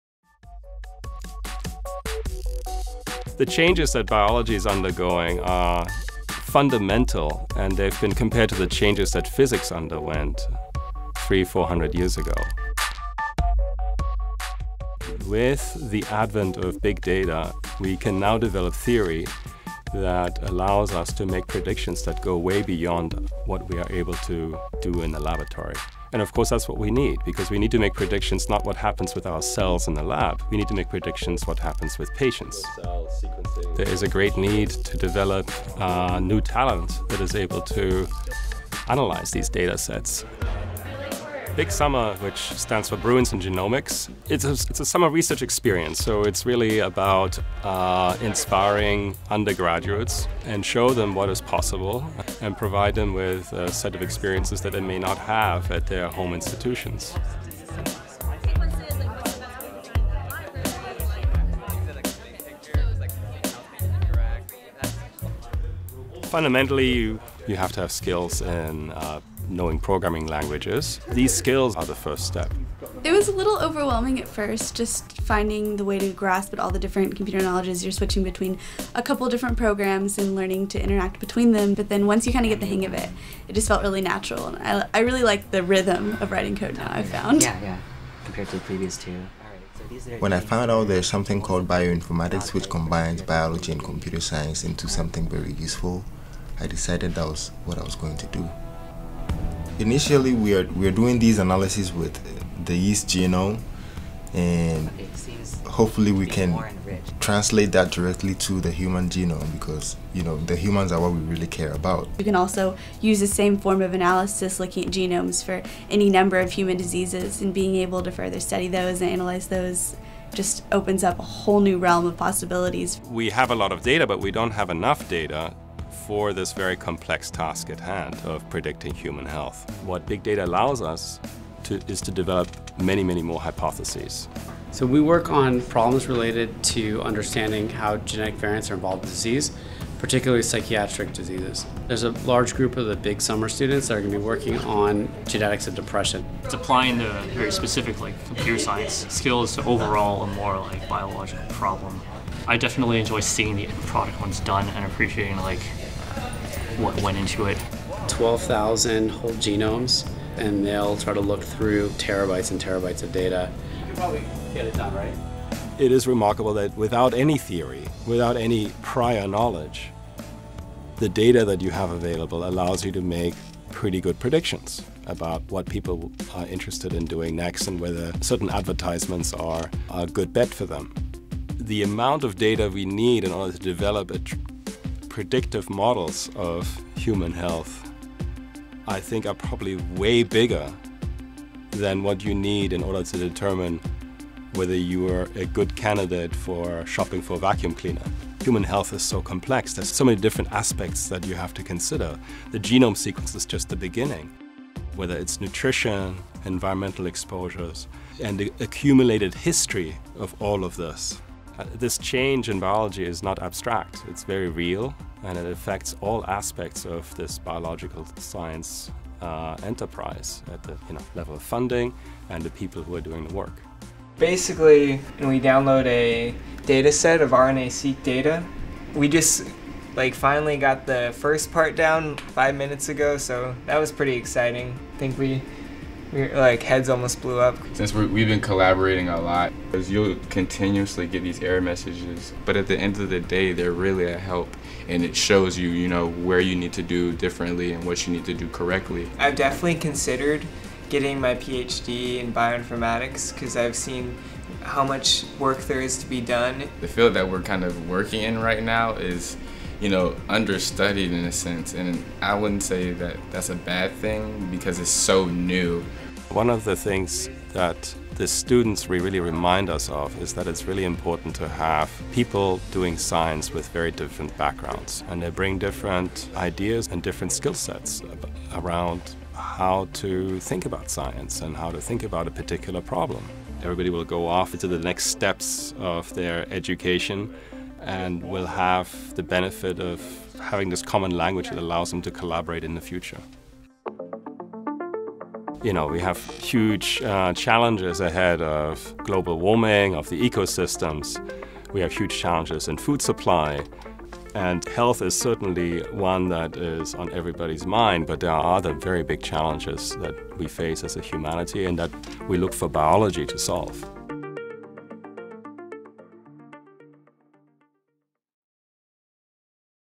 Training future leaders in biosciences
The Bruins in Genomics summer program does more than introduce a national pool of students to the latest data analysis techniques and skills, it also strives to help increase participation of underrepresented students in the sciences.